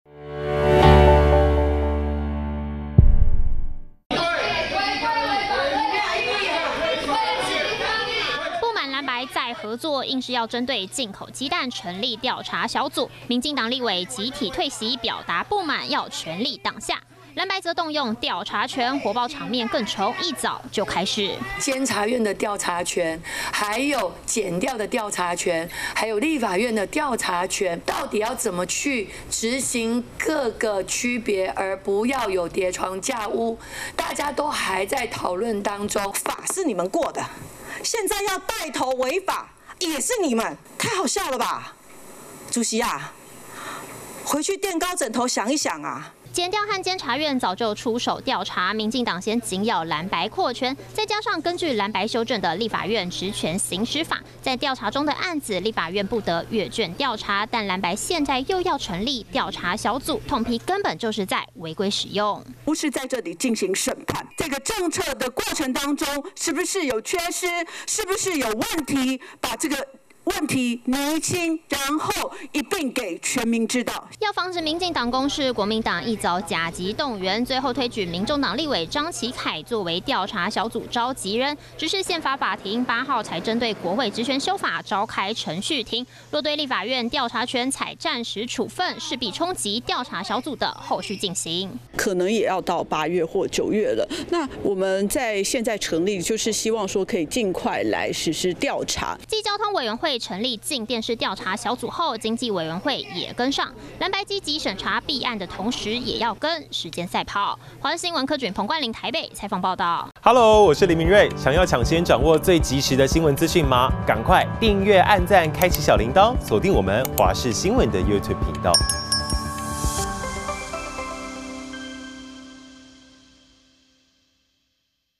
藍白成立進口雞蛋調查小組 綠委不滿「集體退席」｜華視新聞 20240711
立法院經濟委員會今(11)日針對進口雞蛋成立調查小組，引起民進黨不滿，認為成立調查小組的行為違反，藍白自己修正的立法院職權行使法第46條之2，針對調查中的案件不得調查的規定，質疑藍白帶頭違法，集體退席抗議。